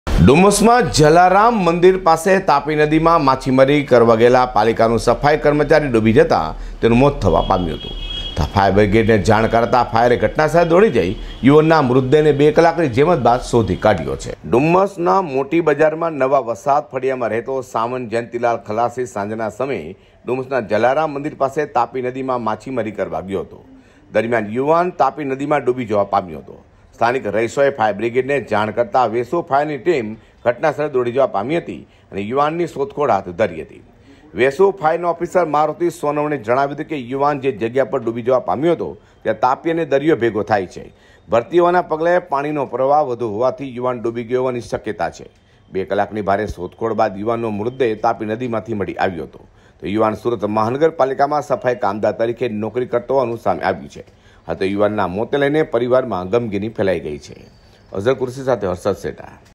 ડુમસ પાસે તાપી નદીમાં માછીમારી કરવા ગયેલો પાલિકાનો સફાઈ કર્મી ડુબી ગયો
ડુમસના મોટી બજારમાં રહેતો પાલિકા સફાઈ કર્મચારીનુ ડુબી જતા મોત થતા પરિવાર શોકમાં ગરકાવ